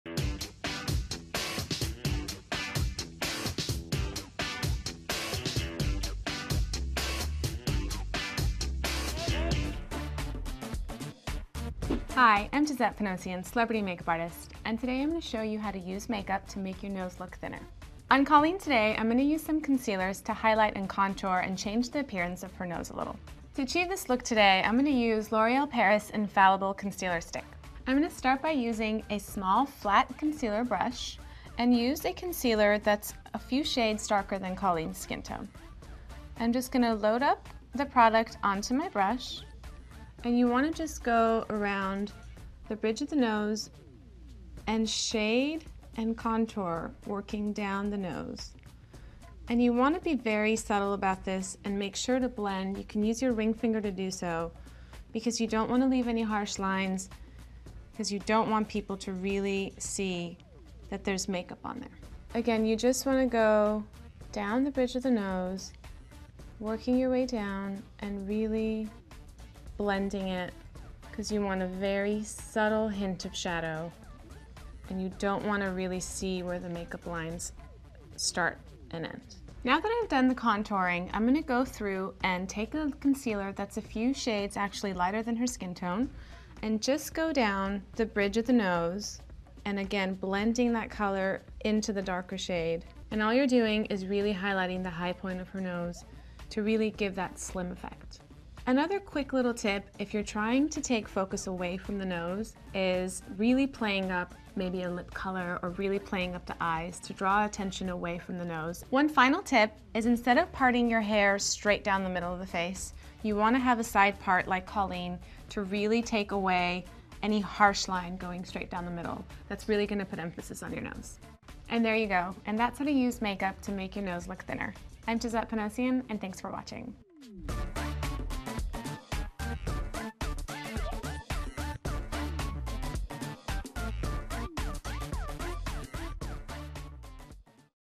Makeup to Make a Nose Look Thinner
Makeup can be used to change the appearance of your natural features, including your nose. Find out about makeup to make a nose look thinner with help from a celebrity makeup artist in this free video clip.